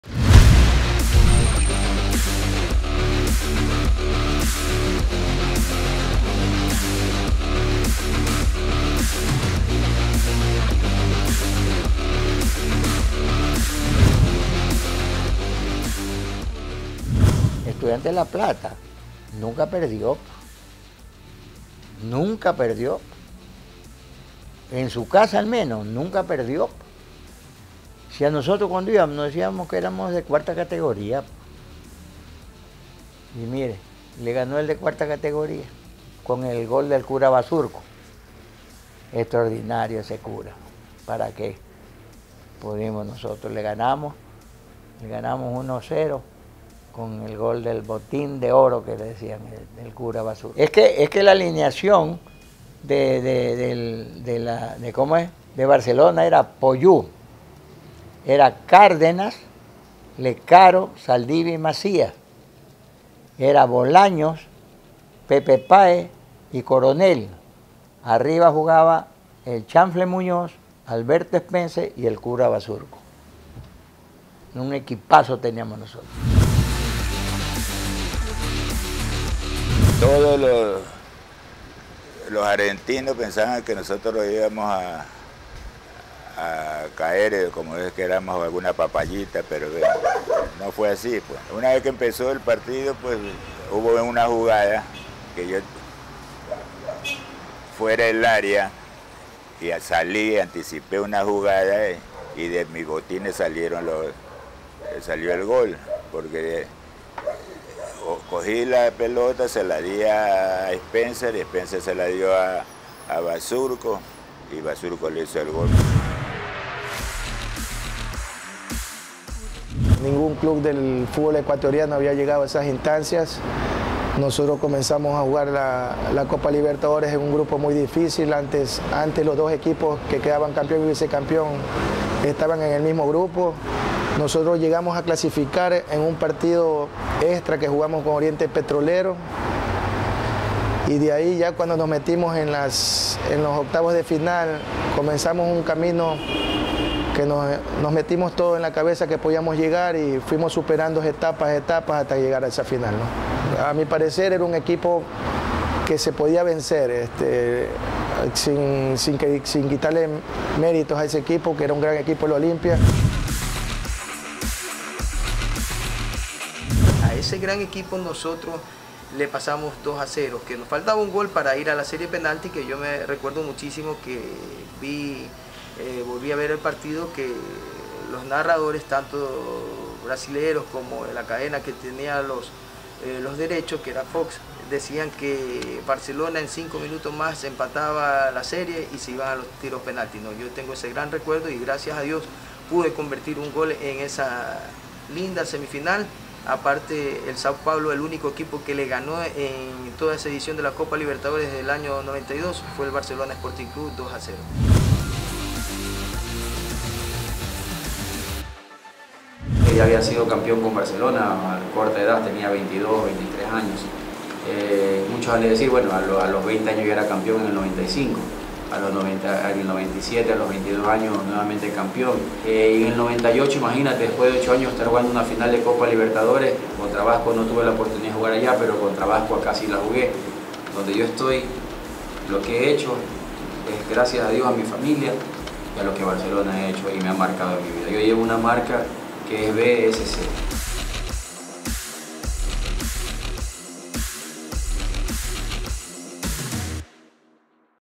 REVISTA BSC - GLORIAS CANARIAS QUE JUGARON EN COPA LIBERTADORES DE AMÉRICA
GLORIAS CANARIAS QUE JUGARON EN COPA LIBERTADORES DE AMÉRICA.
Luciano Macías, Vicente Lecaro, Julio Guzmán, José Gavica y Fricson George.

Entrevistas: Cessia Murillo, Jaime centeno, Madelaine Valle y Wellington Molina.
Video editado por Steven Jiménez.